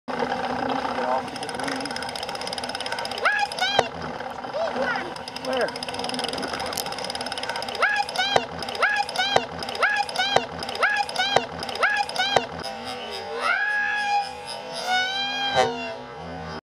Wife Sees Snake
My wife sees a snake...apparently I just missed it.  Turned around and it was slithering through the water.  Looked to be a cottonmouth.